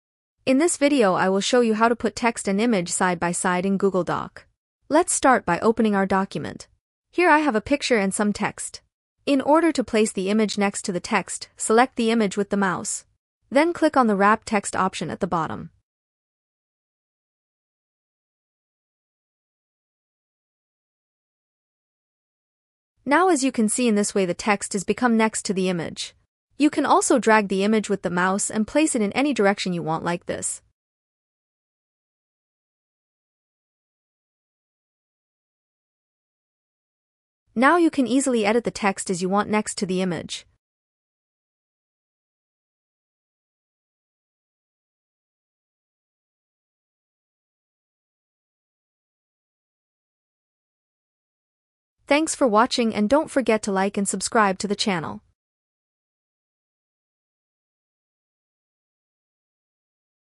How To Put Text And Image Side By Side in Google Doc: Easy Tutorial
Welcome to our easy-to-follow tutorial on how to put text and images side by side in Google Docs!